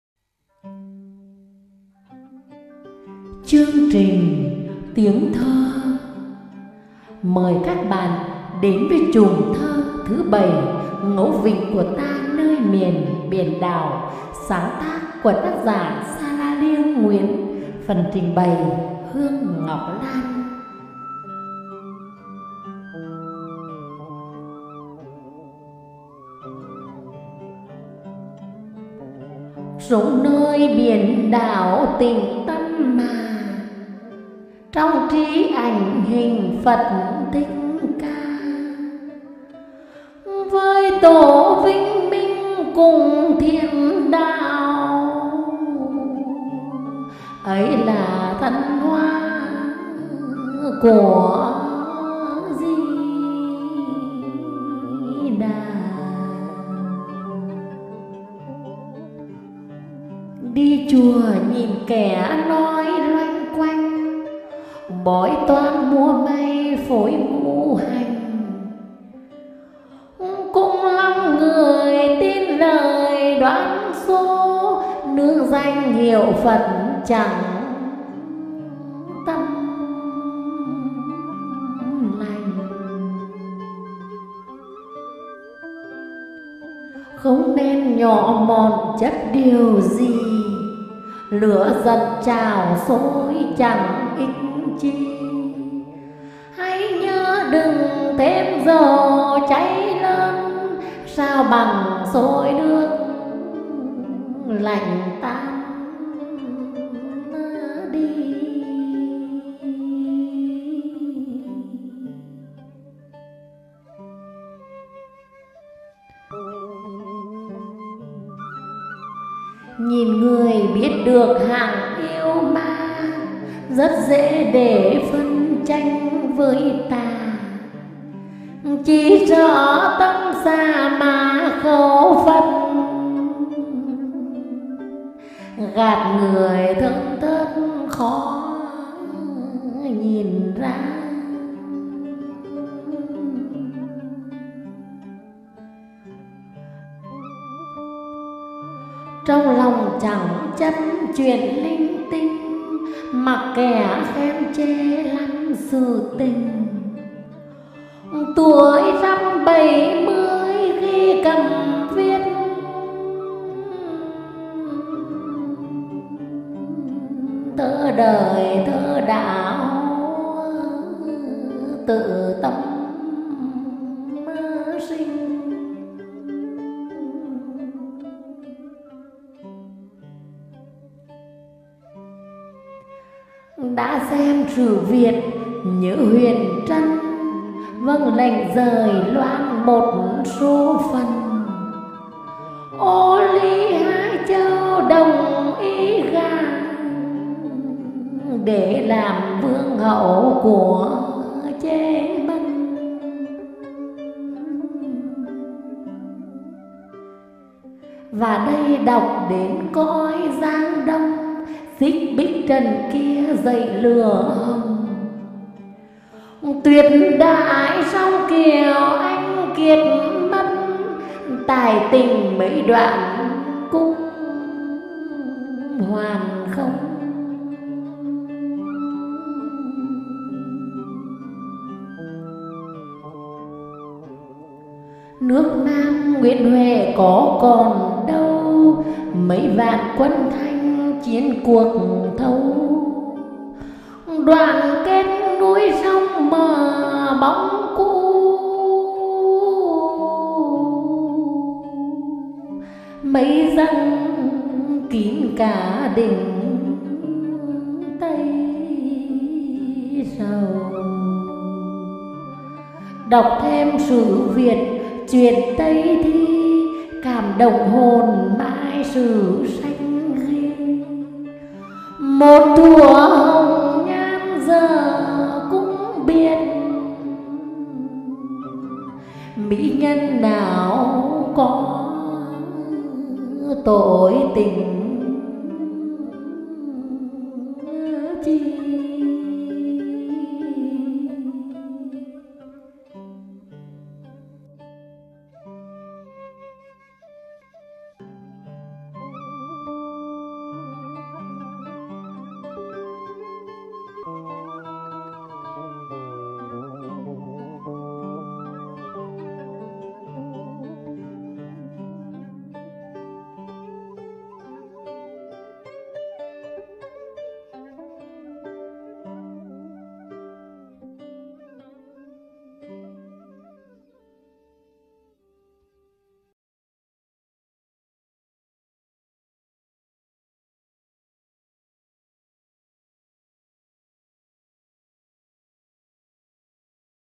NGẪU VỊNH CỦA TA NƠI MIỀN BIỂN ĐẢO Bài 13 14
Thơ: Sarah Lieng Nguyen
Diễn ngâm và dựng Video: Hương Ngọc Lan.